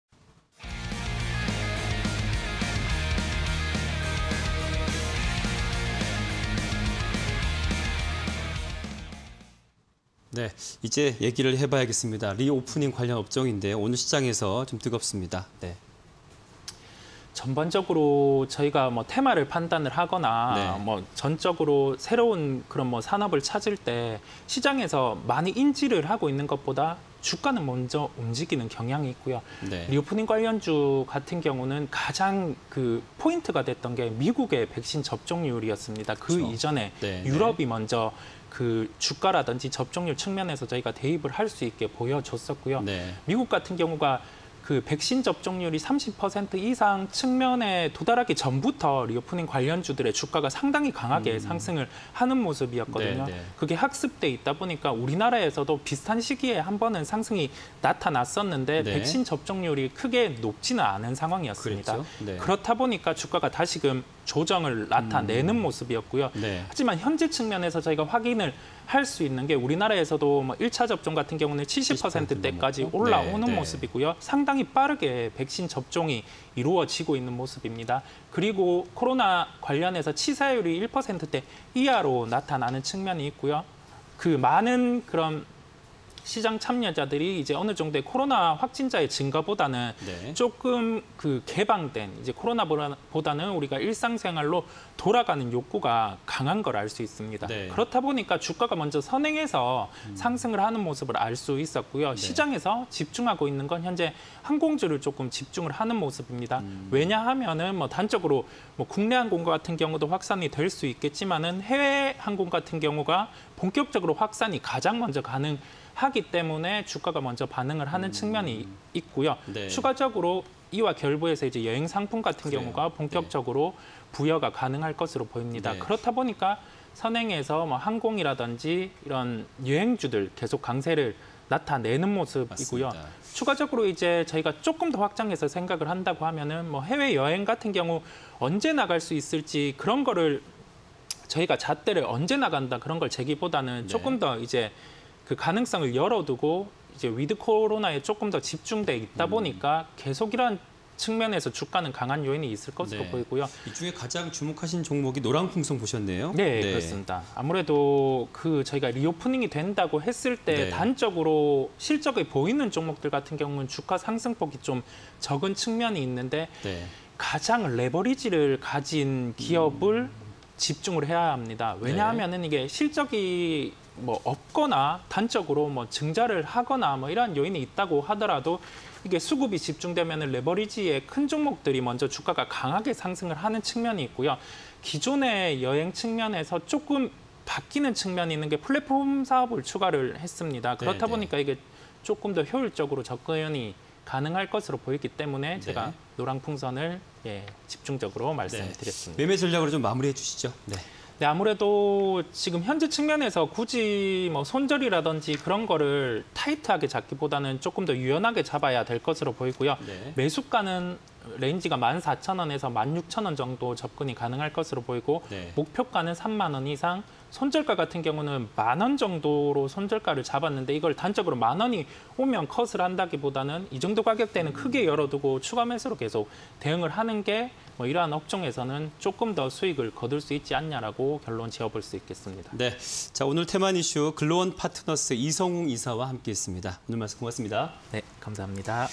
노랑풍선(104620)... 리오프닝 관련주_테마IN이슈 (20210923)
리오프닝 관련주
- 확진자보다 치사율에 주목
- '위드 코로나'의 현실화
- 억제된 수요의 폭증 가능성

노랑풍선(104620)
- 리오프닝 관련 레버리지 큰 기업
- '자유여행' 플랫폼 부각 가능성
- 해외여행 재개 시 추가 상승 예상